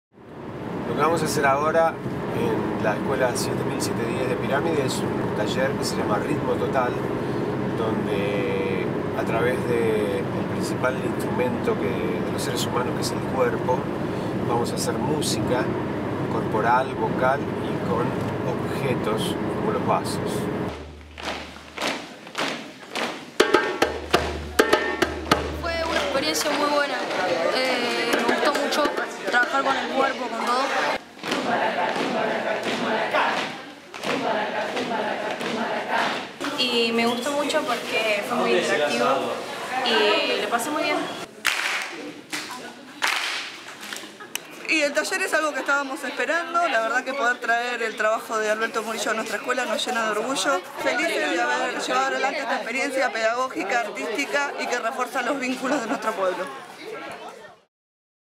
Ritmo Total en Puerto Pirámides
Fluir, sonidos, ritmo: música!
Gran taller en Pirámides con estudiantes y docentes de las Escuelas Secundarias 7710 y 736.

Gracias por crear juntos una maravillosa jornada musical, de expresión y compartir.
Buscar la música dentro nuestro, experiencia única!